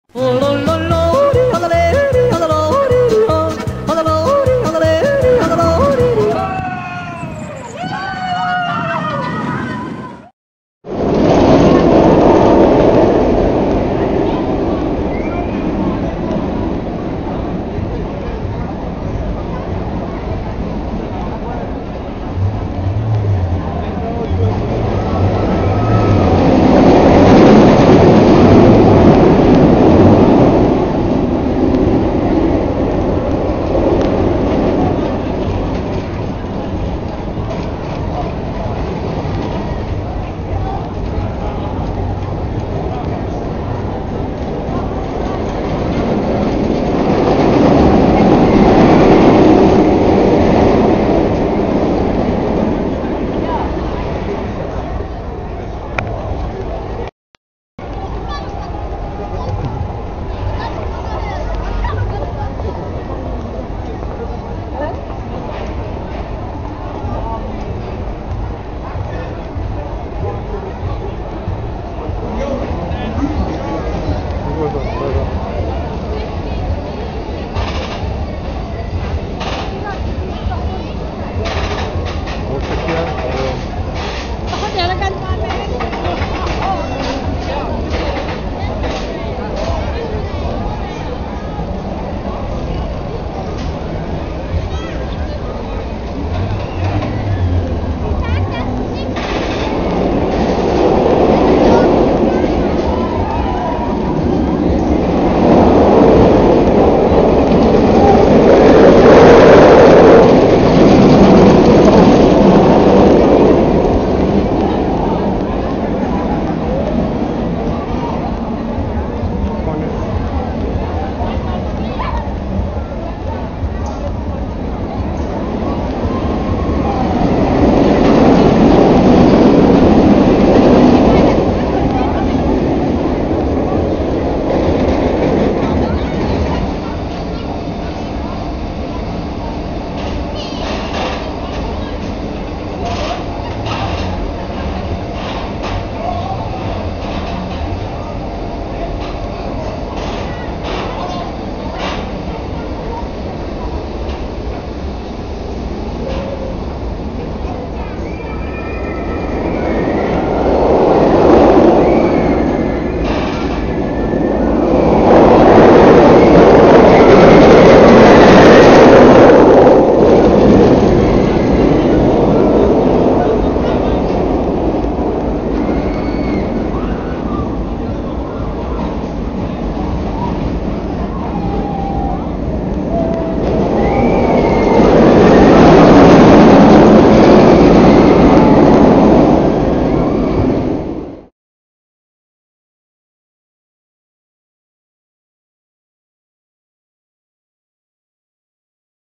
Euro Star /Oscar Bruch - Bremer Freimarkt 2007
Ein wenig Kirmes "Nostalgie." Der ehemalige Euro Star der Familie Bruch auf dem Bremer Freimarkt 2007.
Er war der größte transportable Invertedcoaster der Welt!